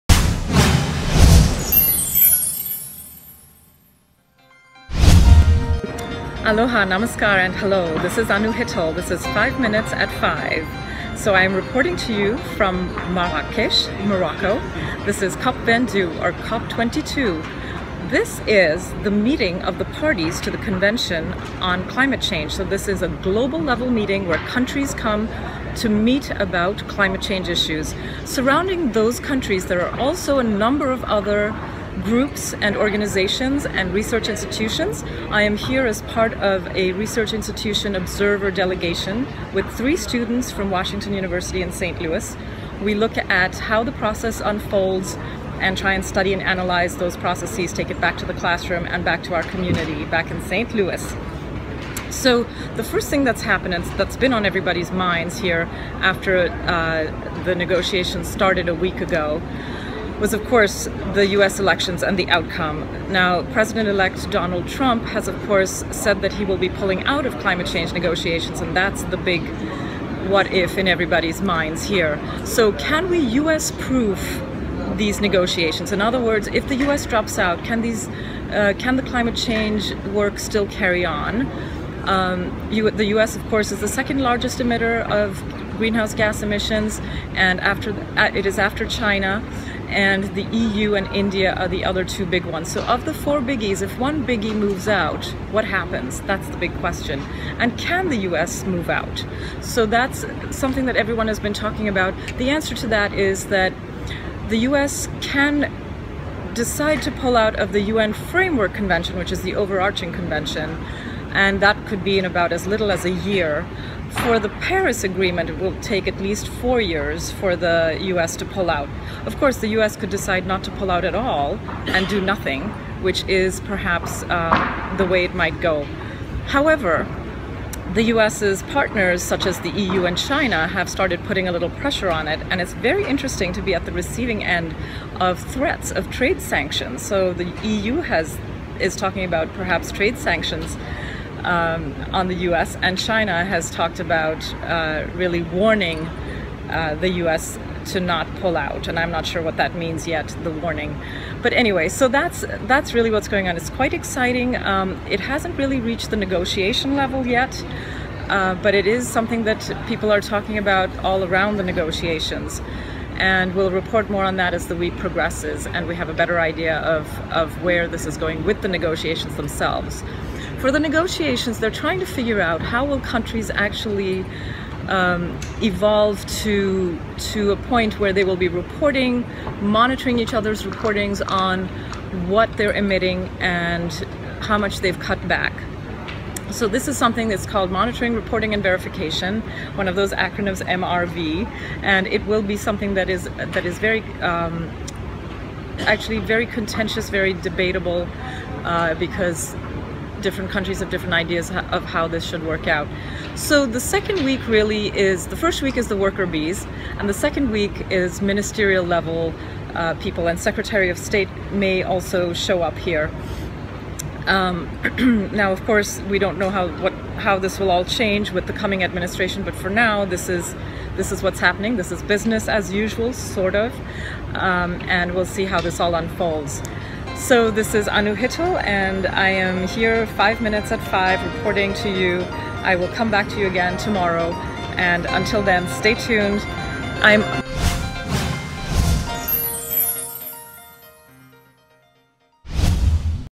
COP22 Coverage - November 15th 2016
Anu Hittle, host of Climate Change -- Beyond Outrage shares perspectives live via Skype from Marrakech COP22.